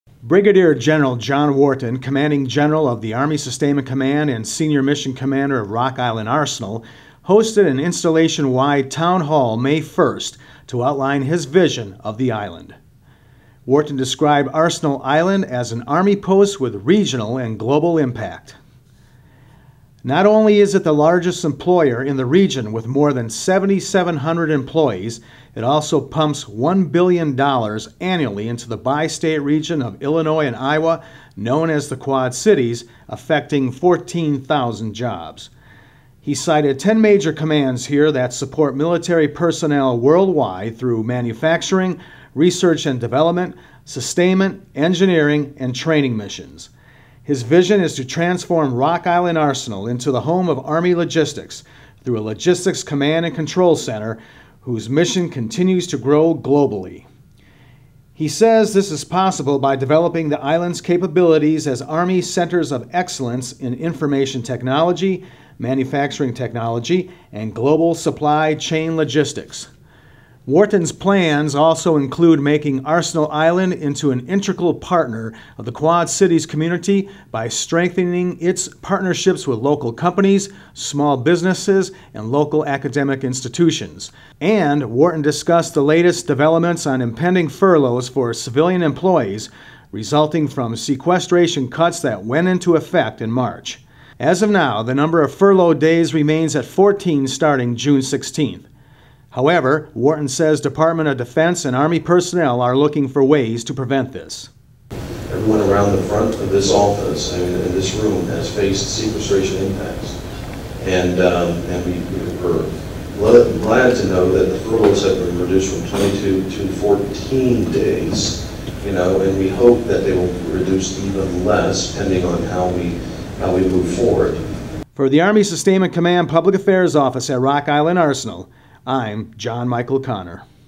RIA senior mission commander holds town hall
Brig. Gen. John F. Wharton, commanding general of the Army Sustainment Command, and senior mission commander of Rock Island Arsenal, hosted an installation-wide town hall May 1 to outline his vision  of the island.